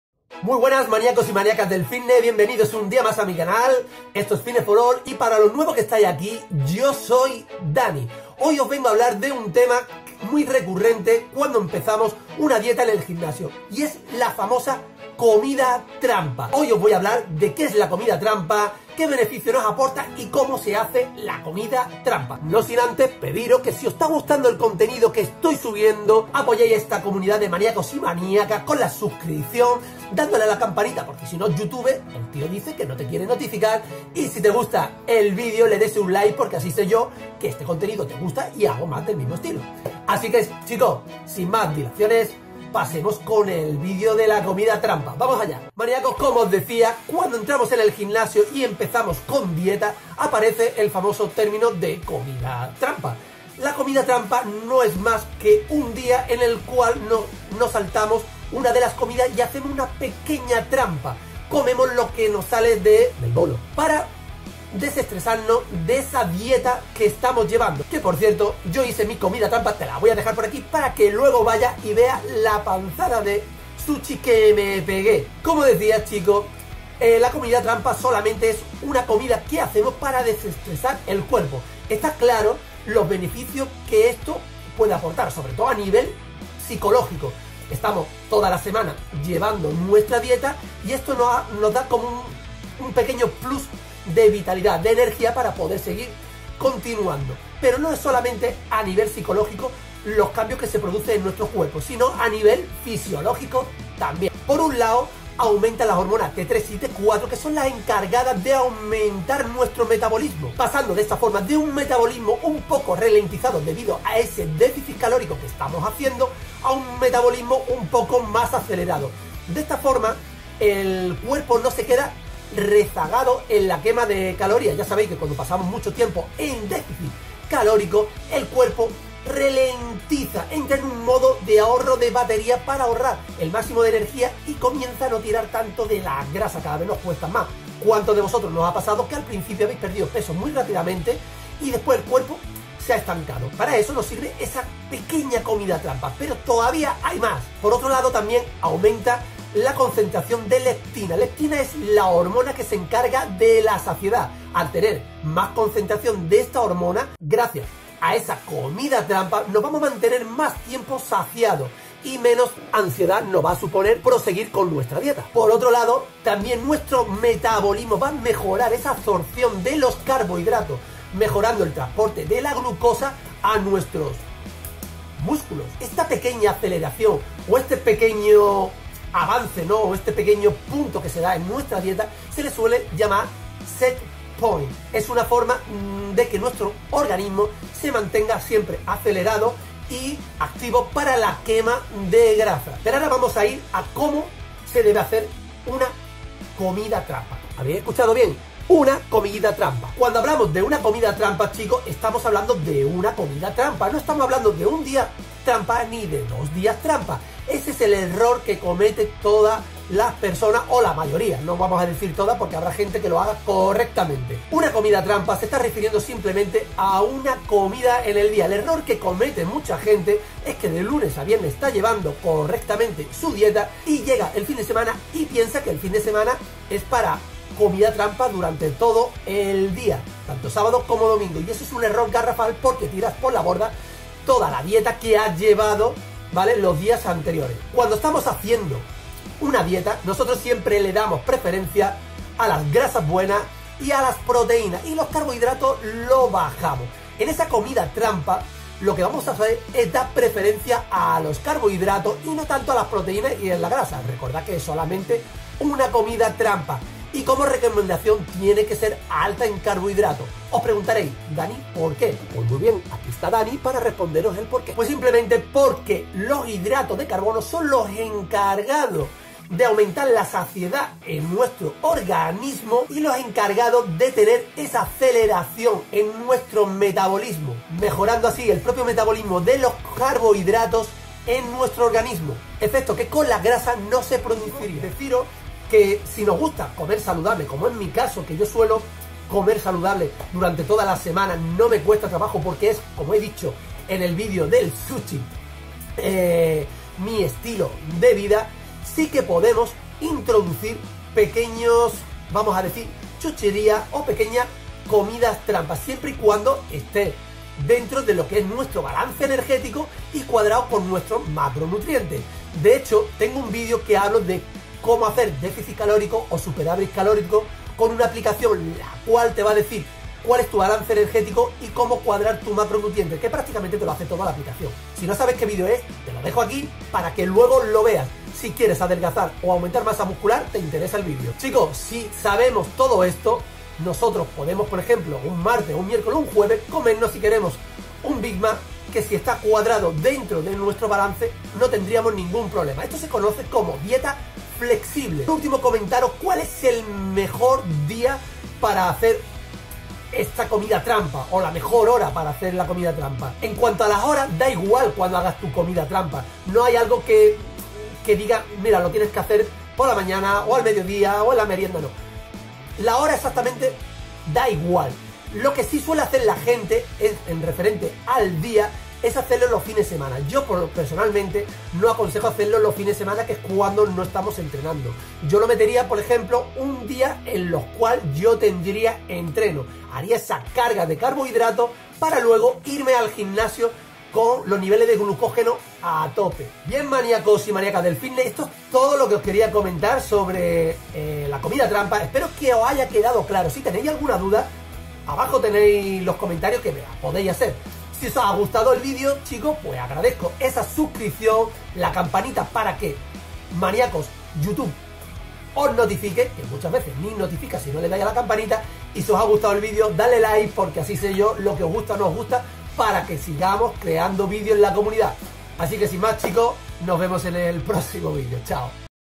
¿Qué es la COMIDA TRAMPA y cómo hacer la COMIDA TRAMPA CORRECTAMENTE?
Maniac@s hoy os hablo de  la COMIDA TRAMPA, como hacer la COMIDA TRAMA, que beneficios trae la COMIDA TRAMPA y cuando deberíamos hacer COMIDA TRAMPA.

Comentaros que para llevar una comida trampar debemos de llevar una dieta, sí no sabéis como llevarla os recomiendo mi video del DÉFICIT CALÓRICO y SUPERÁVIT CALÓRICO:

Maniac@s! quiero que me dejéis vuestro comentarios sobre vuestra experiencia con la COMIDA TRAMPA!